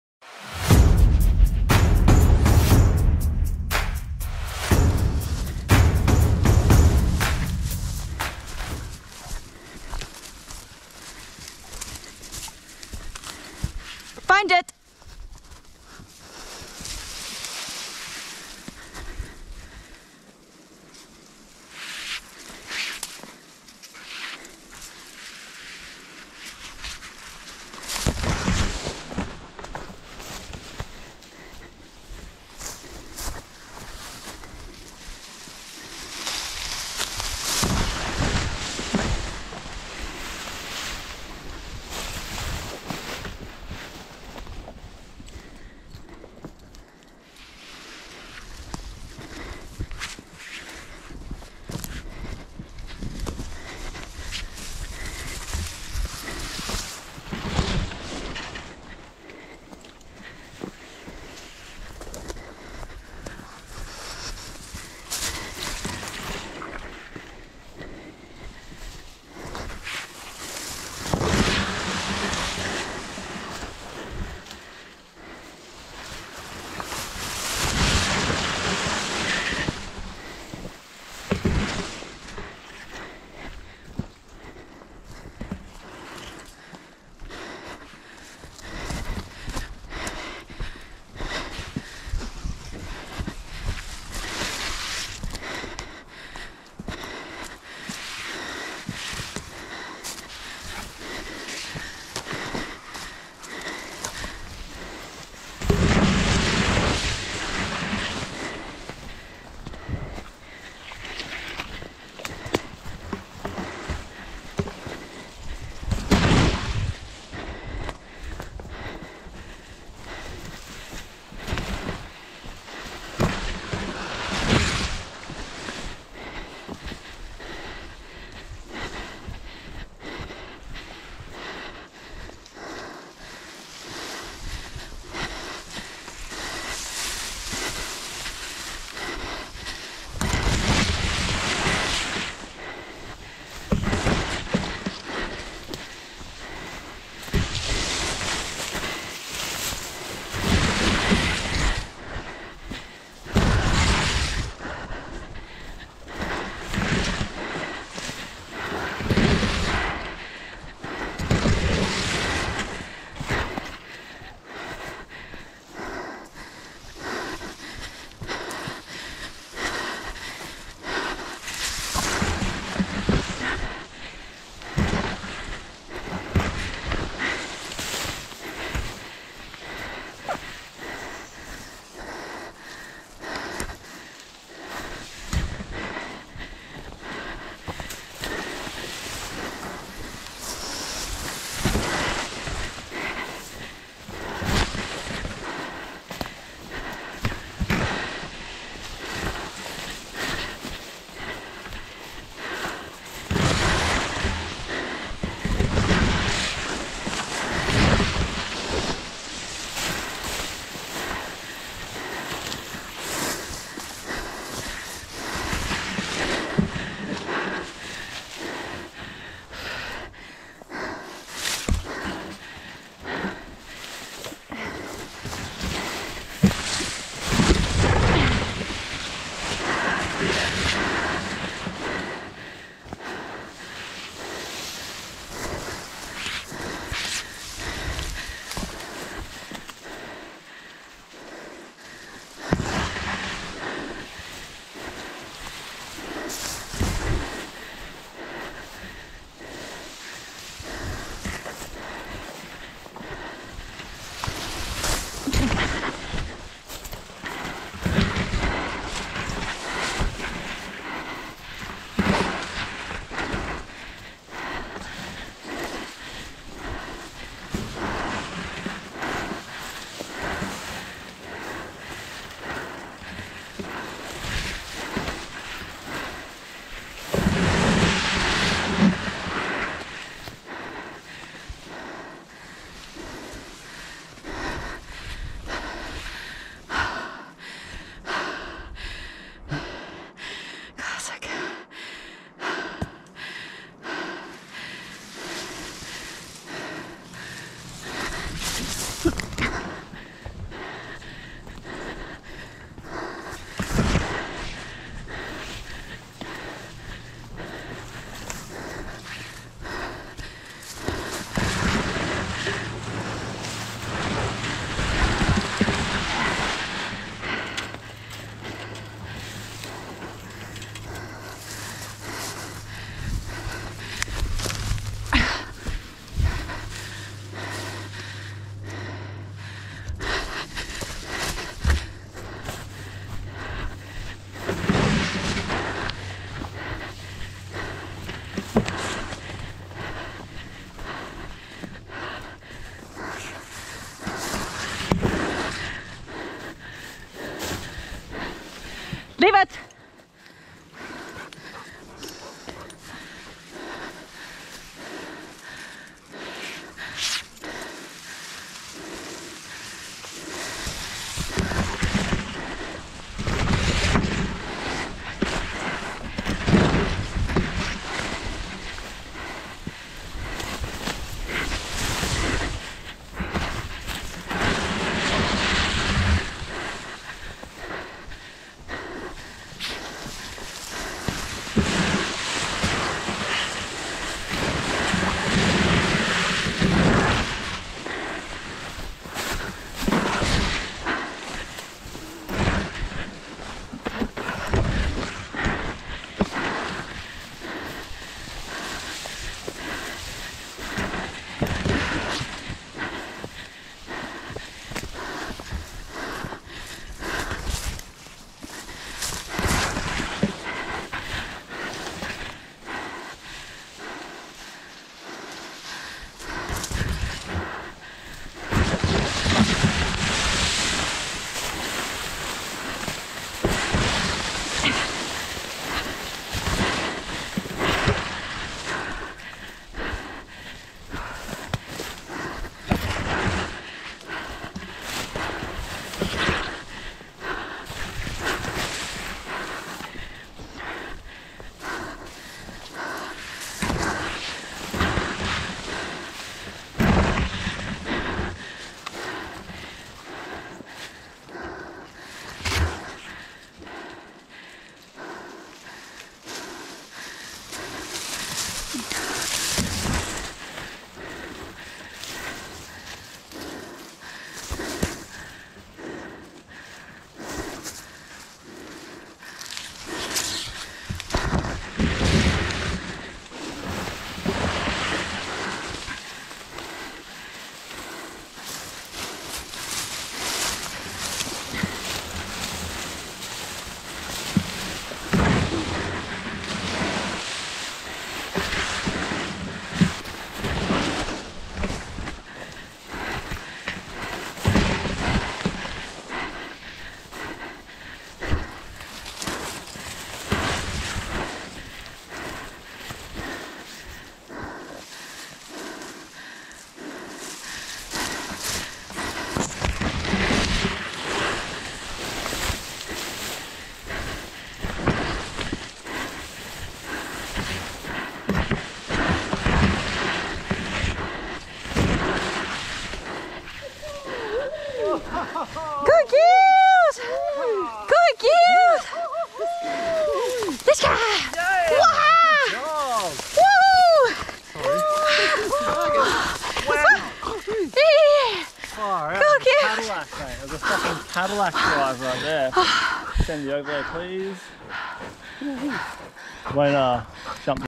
GAK9 Australia - Liska - Field Handler Certification
Dog handler Alice and K9 Liska

Runner - Exra King
Track Length - 510m
Track Age - 33min
Trail Time - 9min 45sec
GAK9 Trainer - Jeremy Wikner
Date - 24 Mar 24
Location - Waiuku Forest, New Zealand